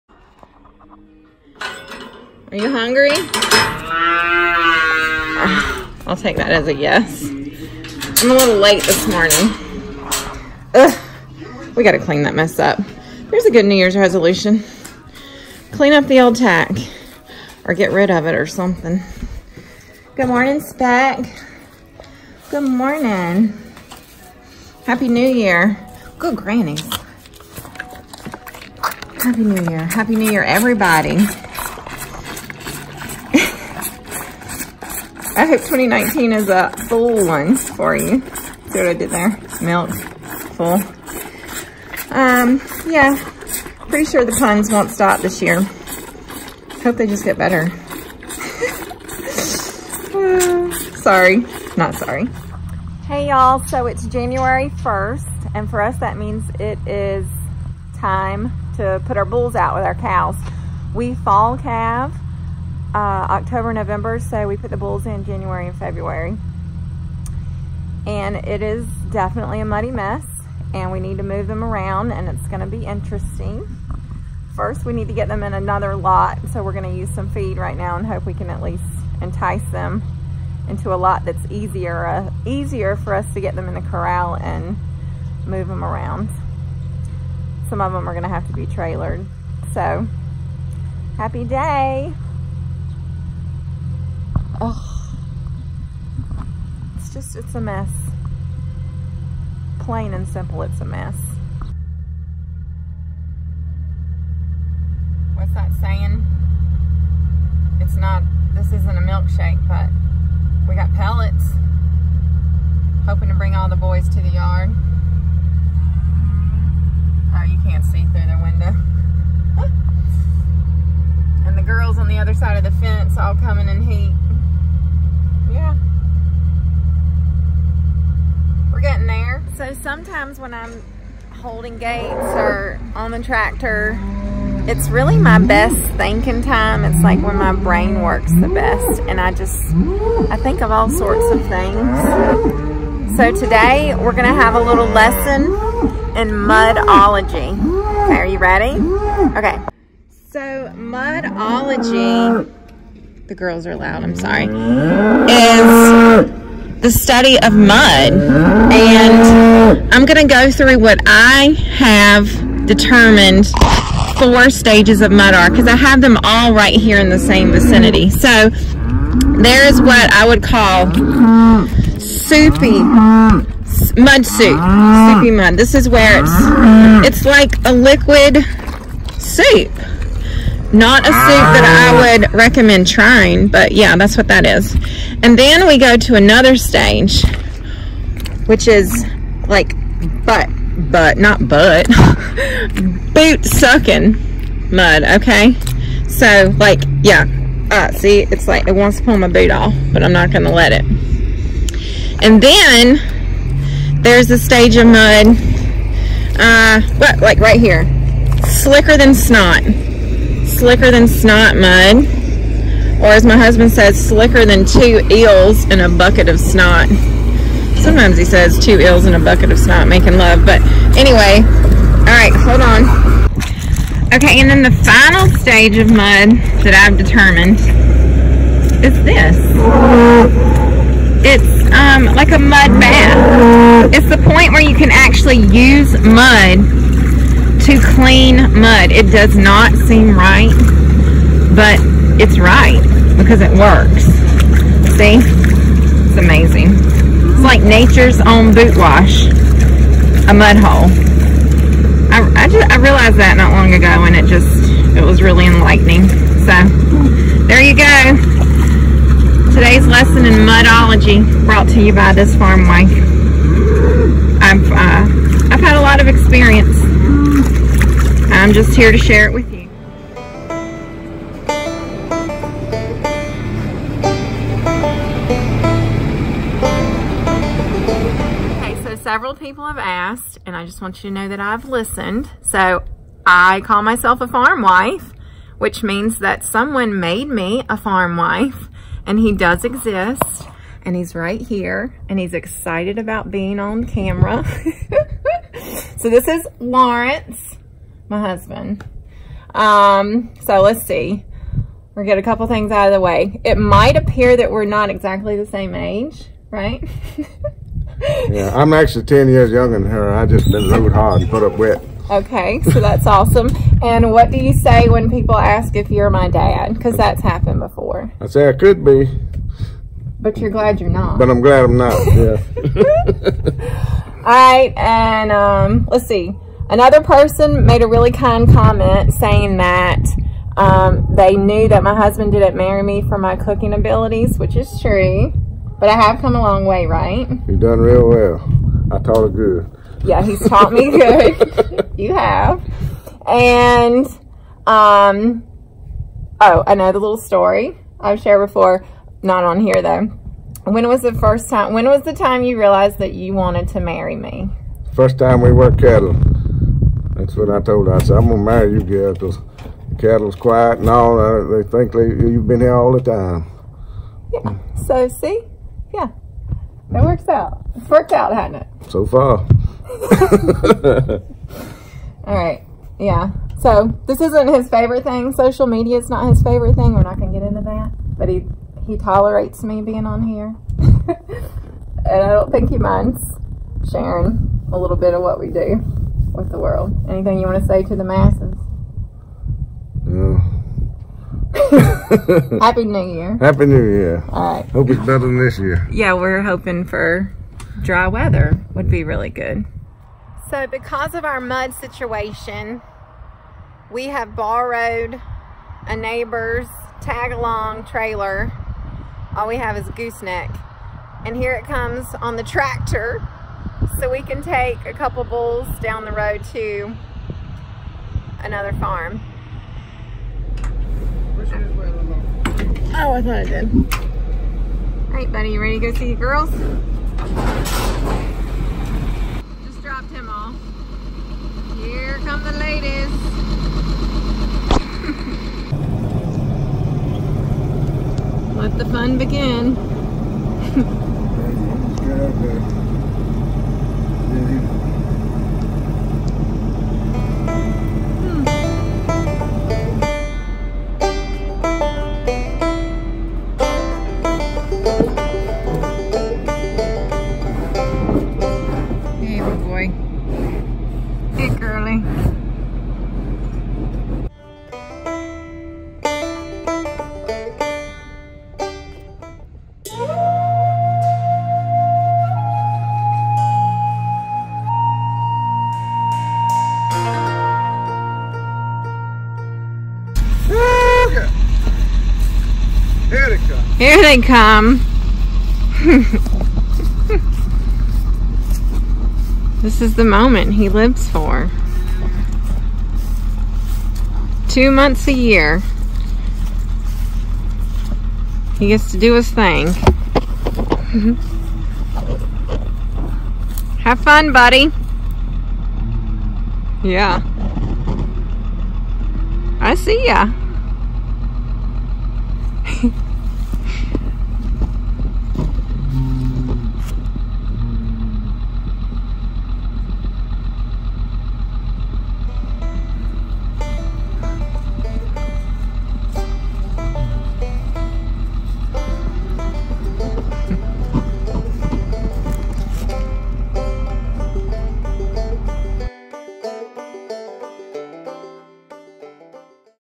The Bulls Are Out & A Lesson in Mudology
Letting the bulls out to do their thing and sharing a lesson in "mudology." Also...the farmer does exist (4:35). Happy New Year, y'all!

MY FAVORITE THINGS ➽